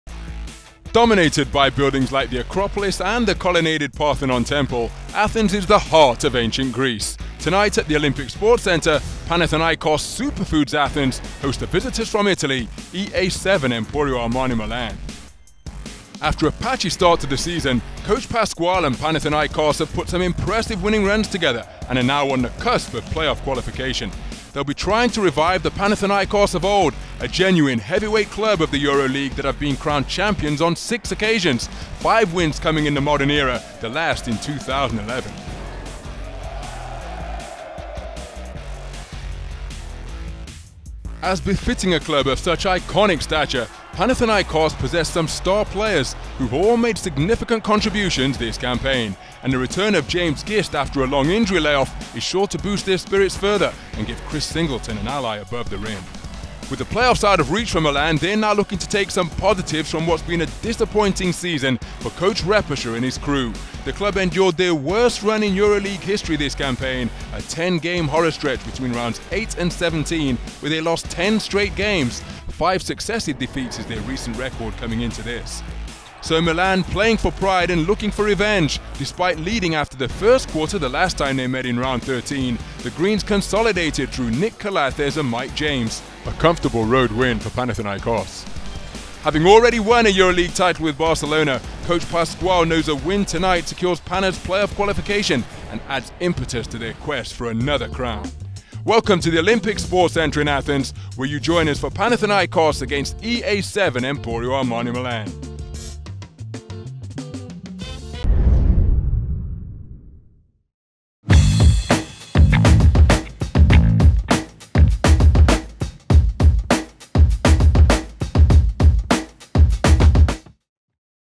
GameON trailer: Panathinaikos Superfoods Athens-EA7 Emporio Armani Milan
As Panathinaikos Superfoods Athens and EA7 Emporio Armani Milan get ready for battle, it's the perfect time for a sneak peek at their Turkish Airlines EuroLeague showdown later tonight! Watch it live on Euroleague.TV! Get your EBTV pass now!